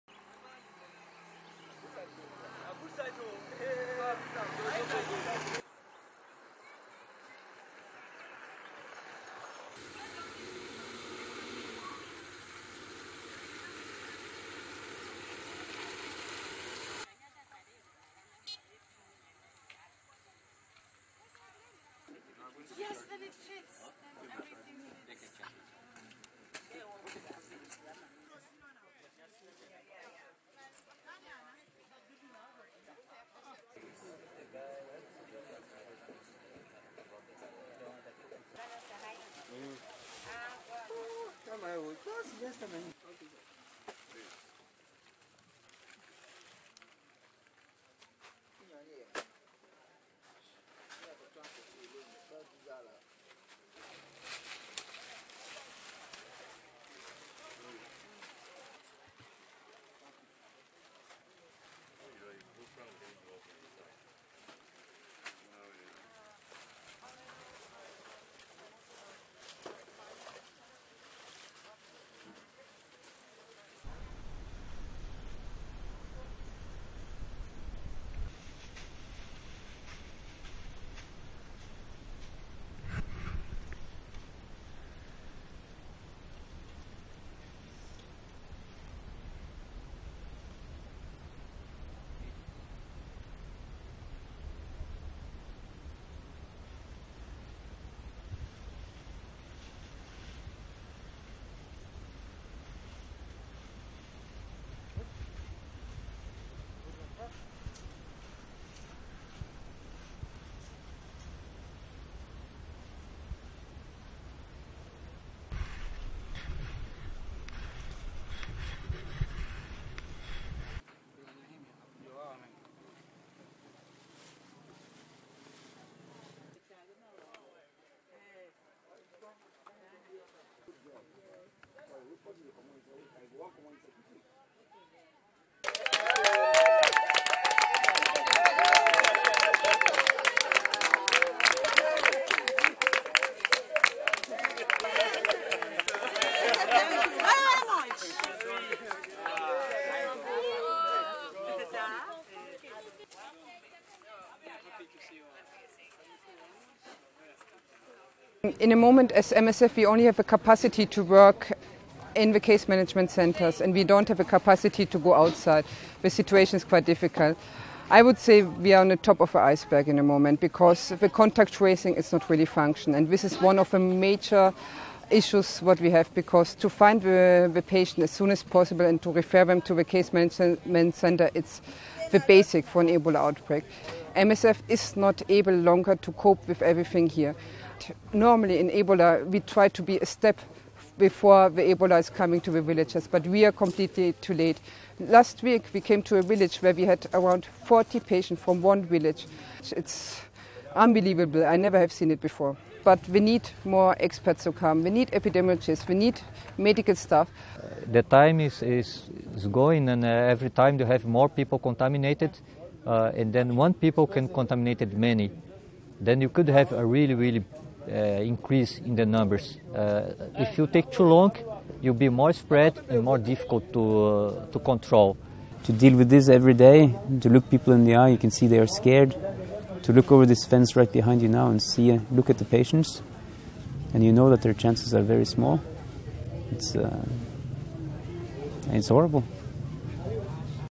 Ebola | A race against time to control the outbreak
Friday 11 July 2014. Over the last two weeks, Médecins Sans Frontières/Doctors Without Borders (MSF) has treated more than 70 patients with symptoms resembling those of Ebola in Kailahun treatment centre, eastern Sierra Leone. MSF is concerned about a possible increase in patients in the coming weeks, as teams on the ground are starting working to find people sick with the virus.